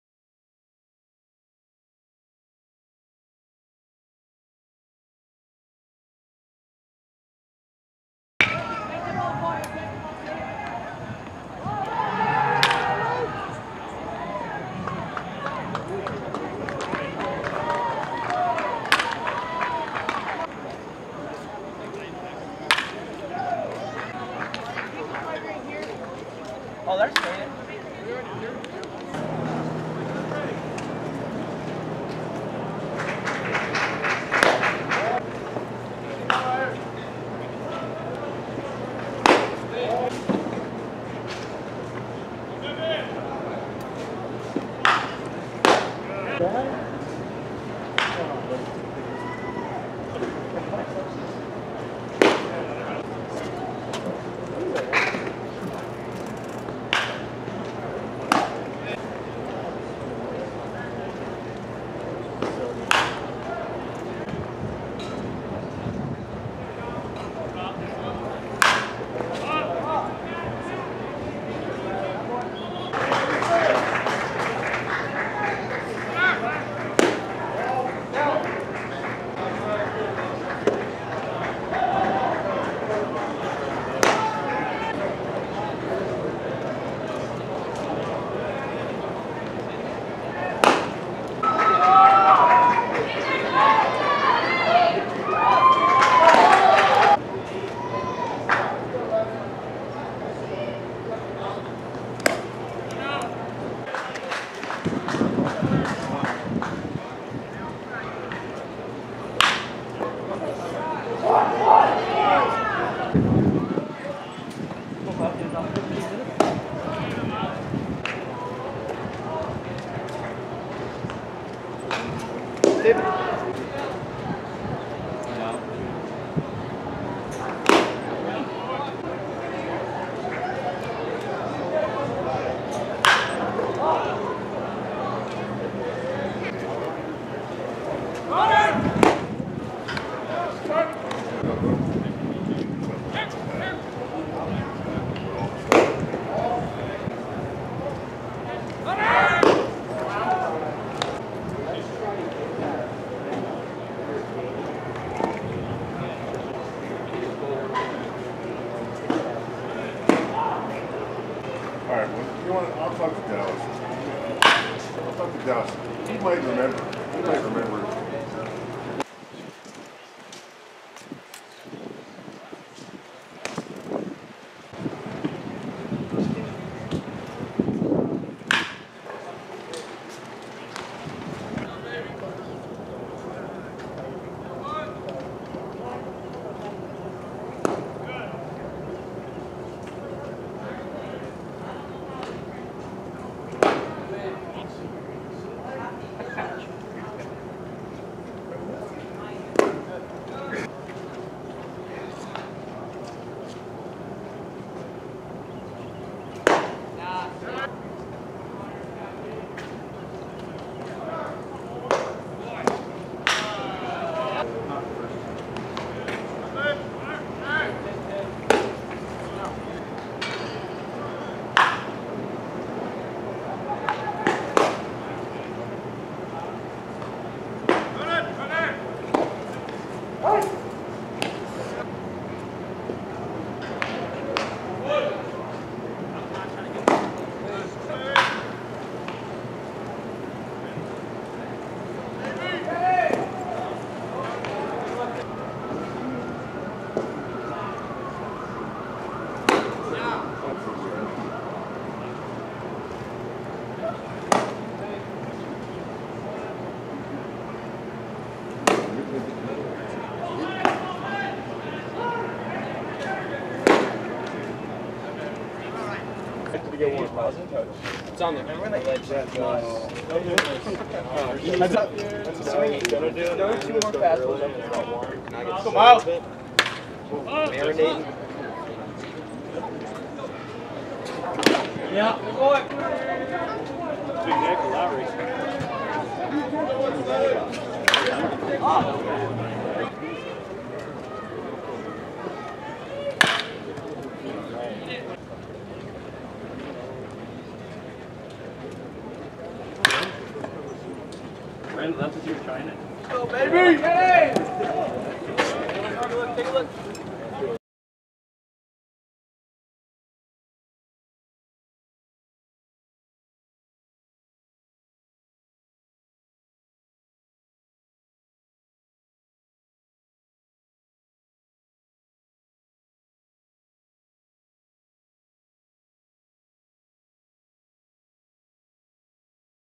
Myles Smith (2020-8-6-10) at the Area Code Games (Emerson, GA)
Myles Smith (OF, Crossroads HS (Concord, CA)) at the Area Code Games (Emerson, GA).